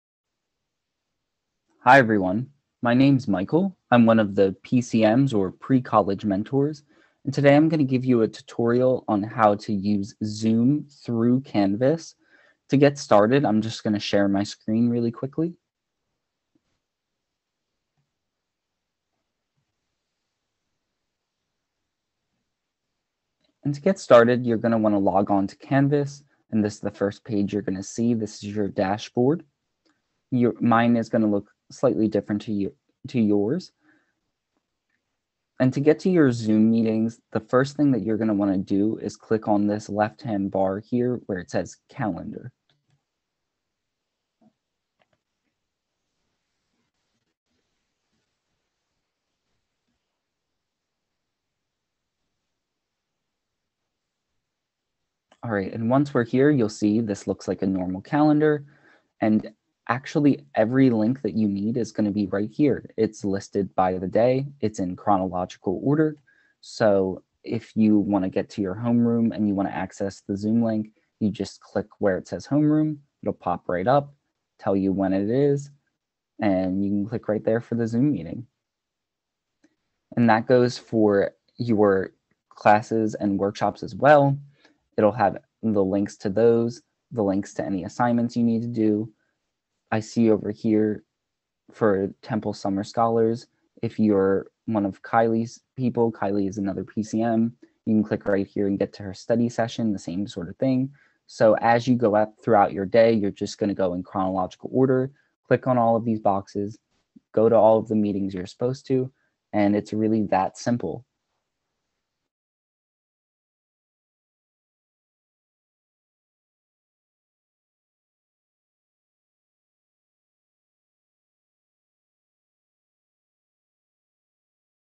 How to Join Zoom Meetings Through Canvas
A tutorial to join Zoom meetings through Canvas for Pre-College students.